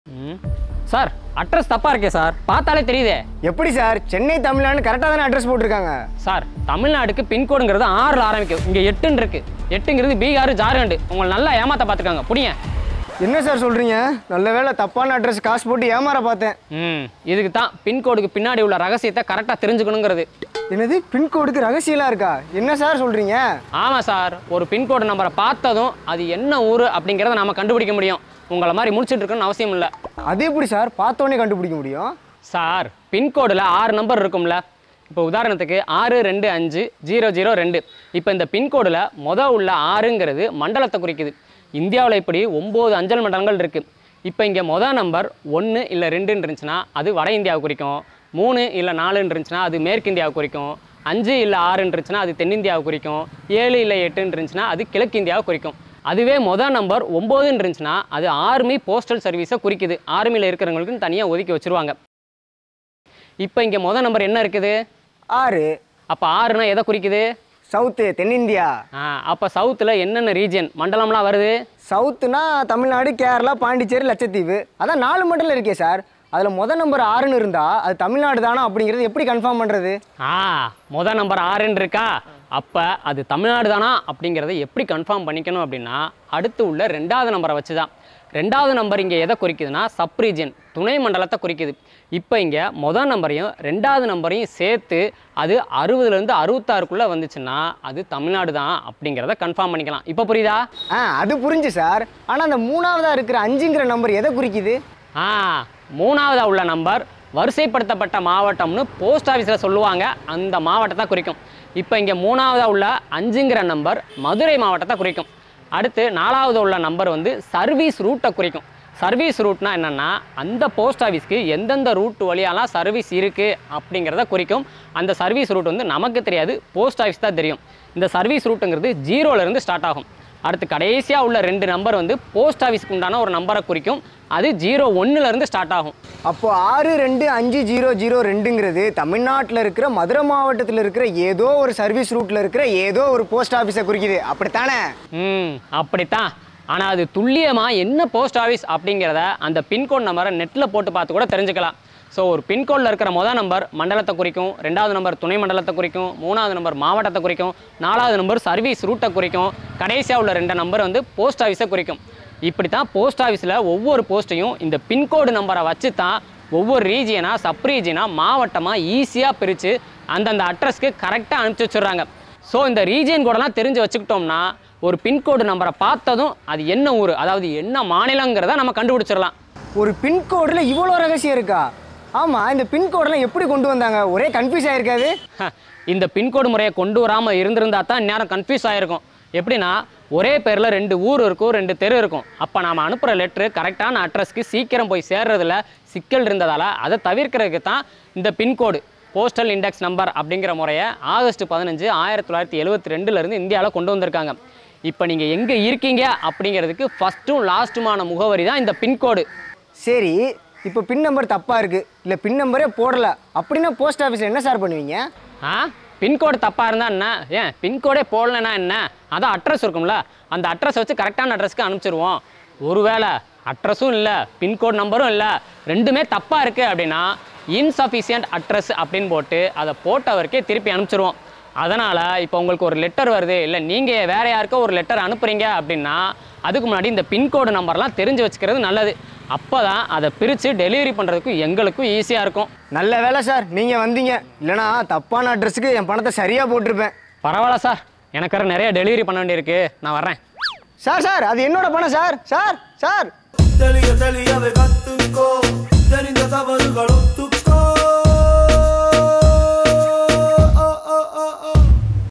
How to Find ADDRESS by PINCODE? | பின்கோடு-க்கு பின்னுள்ள இரகசியங்கள் என்ன? | Decoding of PINCODE
PINCODE வச்சு ADDRESS கண்டுபிடிப்பது எப்படி?

Script & Starring: Kalidass & Sathish
Shot by Ram & Gandhi
Edit by Ram